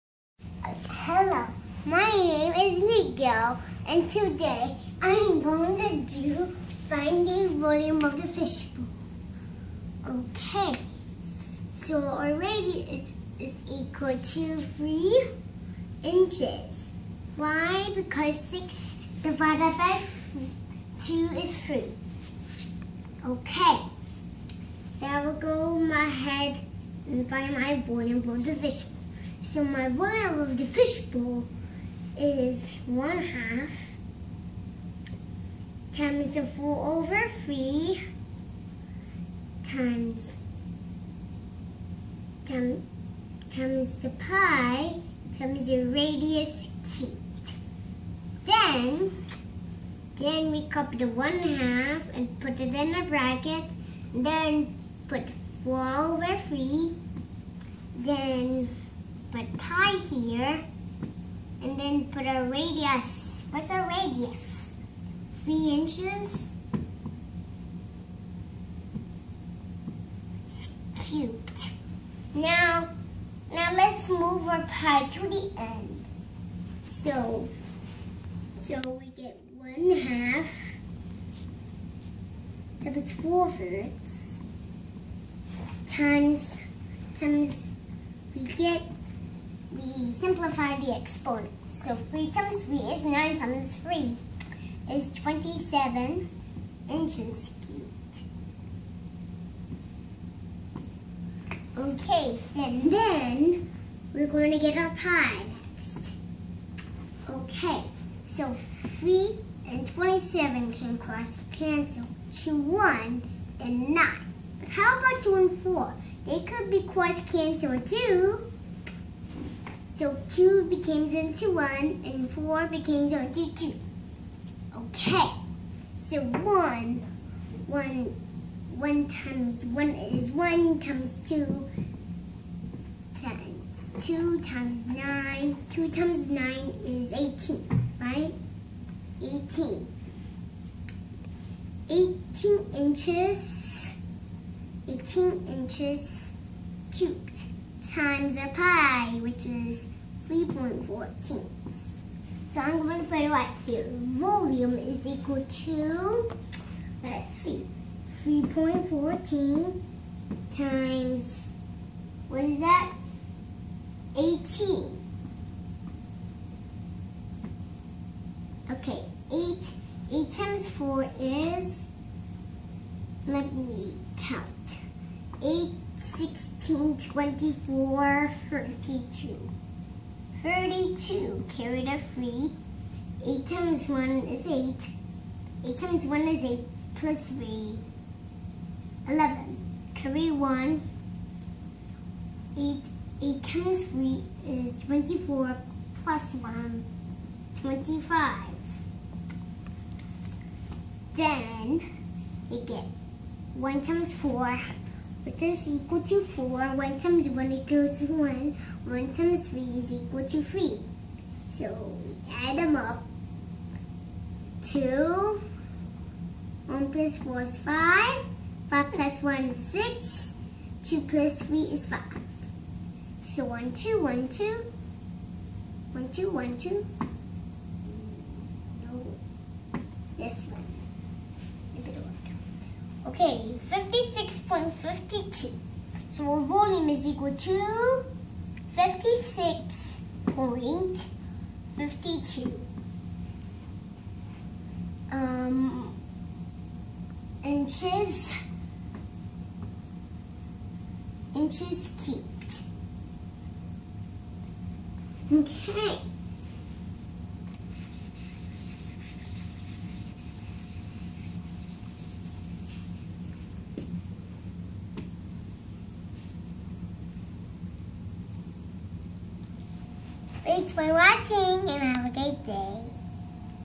Geometry: Volume of the Fish Bowl(Hemisphere)
In Geometry, Hemisphere is exactly half of a sphere. In this video, the fish bowl represents hemisphere where in the formula used is half of that of sphere. Here's Miko @ 4 years old Calculating for the Volume of the Hemisphere.